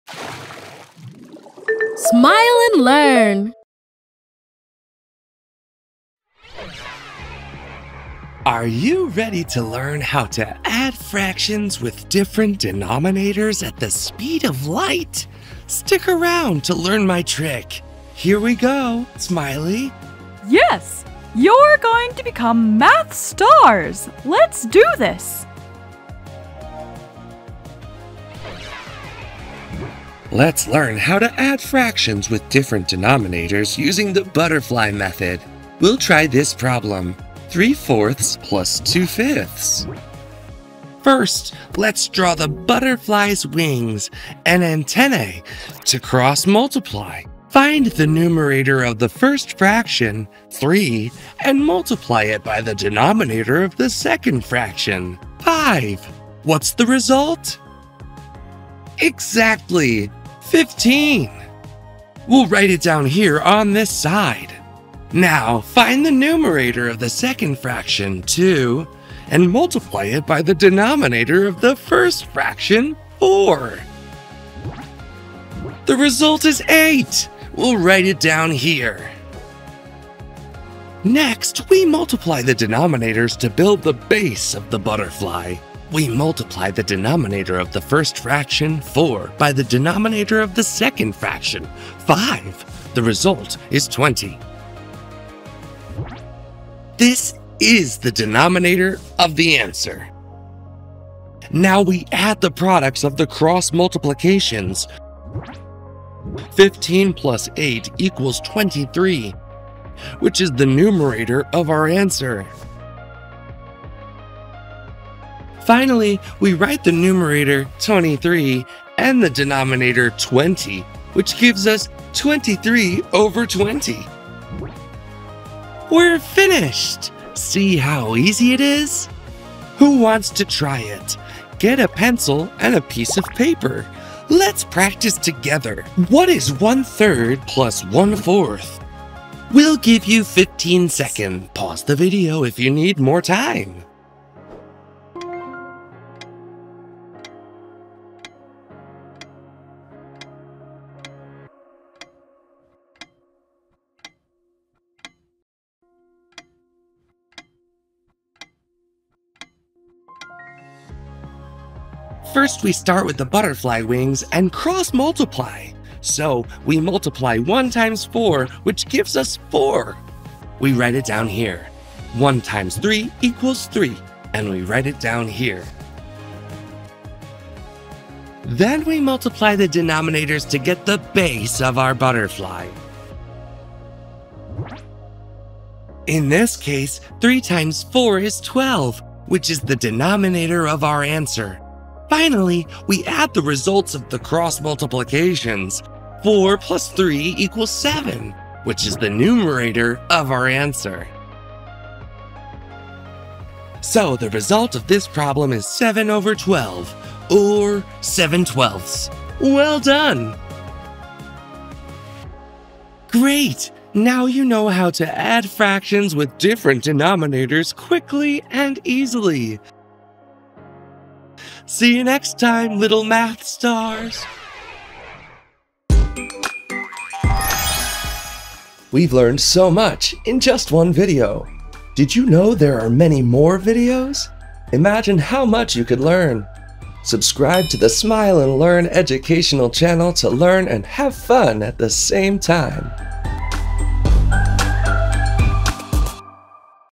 TRICK to ADD FRACTIONS FAST 🔢 The Butterfly Method 🦋 Math for Kids 🚀
Educational video for kids that talks about how to multiply fractions in a simple way with the butterfly method. Our math astronaut will teach you to add fractions with different denominator in a fun and visual way in three simple steps:

🦋 Step 1: Draw the butterfly's wings and antennae by doing cross multiplication
🦋 Step 2: Build the base of the butterfly by multiplying the denominators
🦋 Step 3: Add the results and get the final numerator and denominator

This video is a very useful and interesting resource for children. This trick makes math problems easier and helps to get the result faster. It is an excellent video for elementary school.